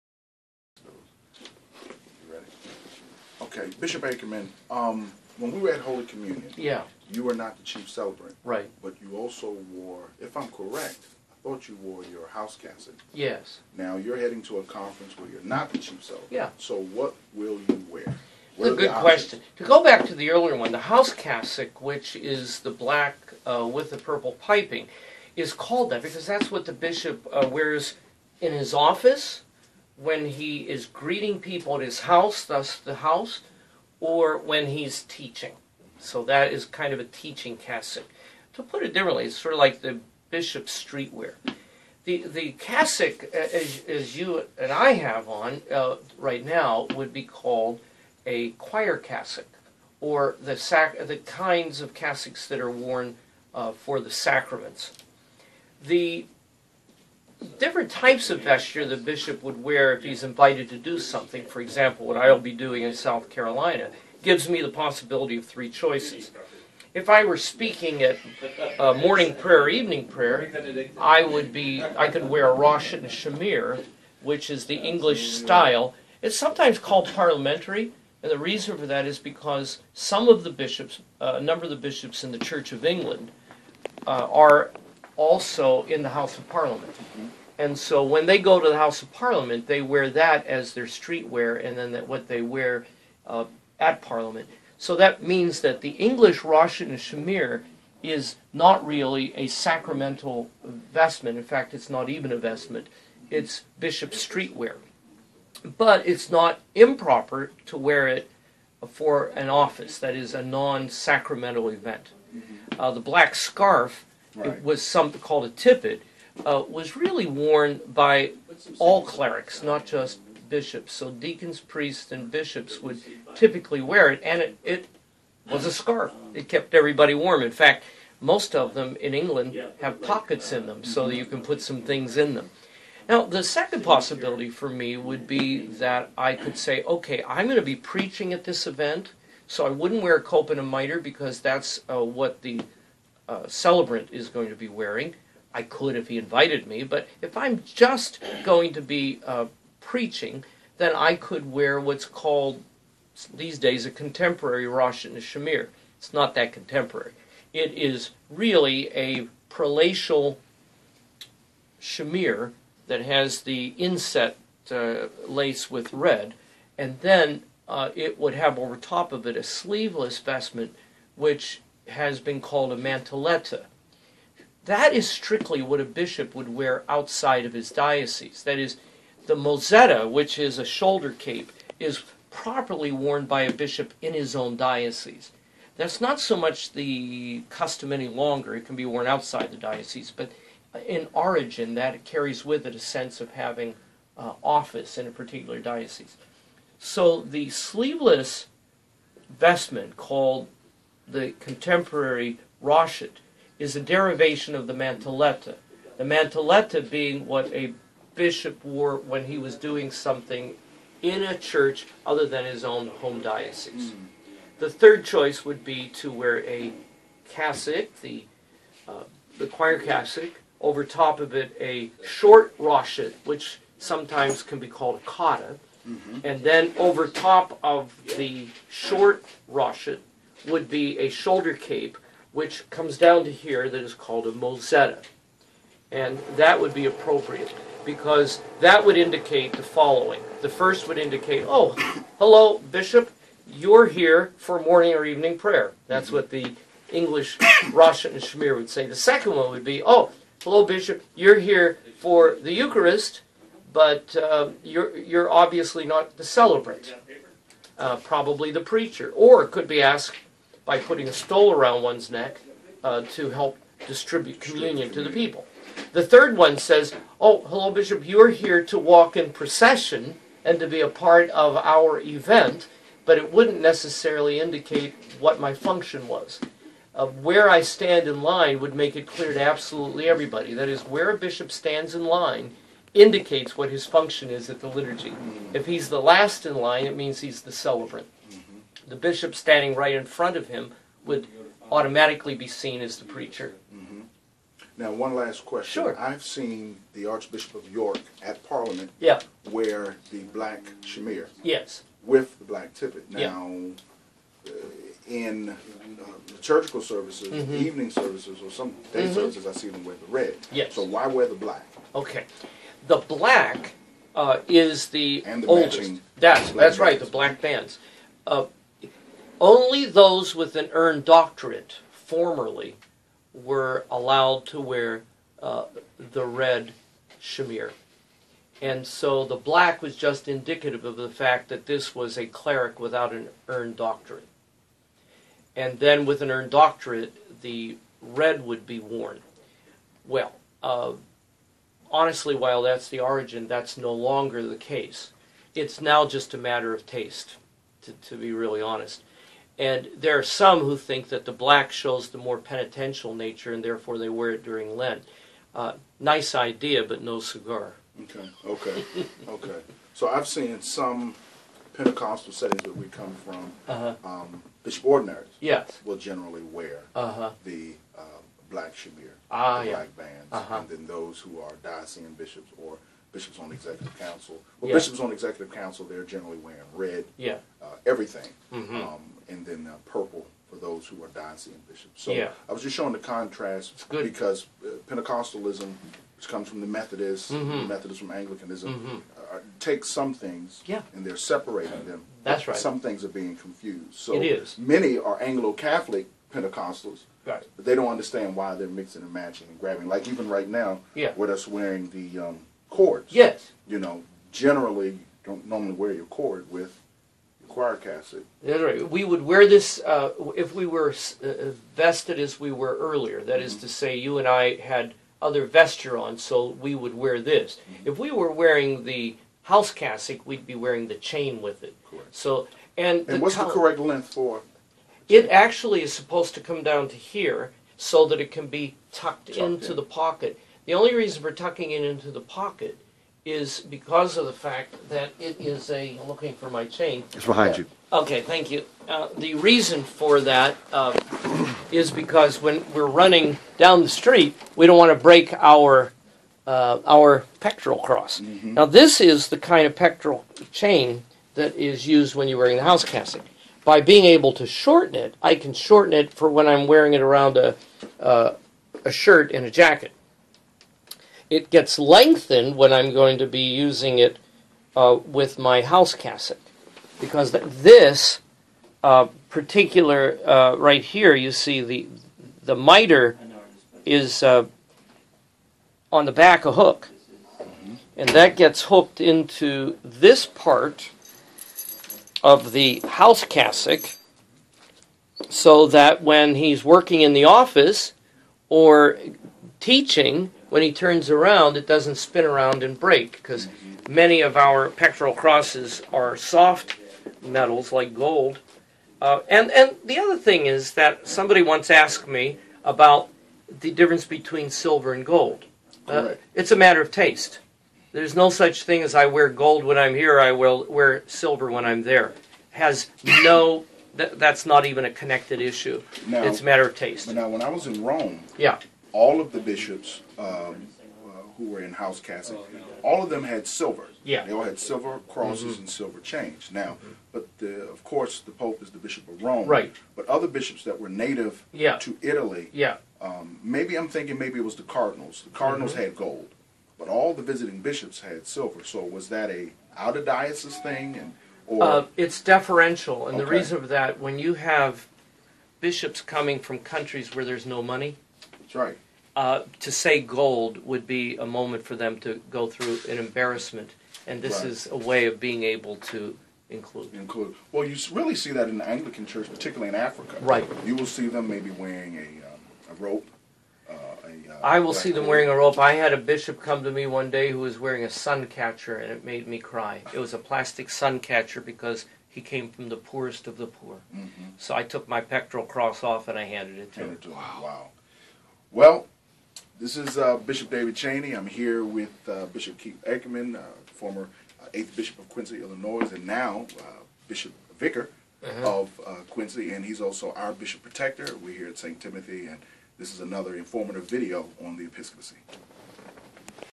What Cassock should a Bishop wear?
Bishop David Chaney takes a few minutes after Mass to ask Bishop Keith Ackerman a questions regarding the proper vestments a Bishop would wear and when.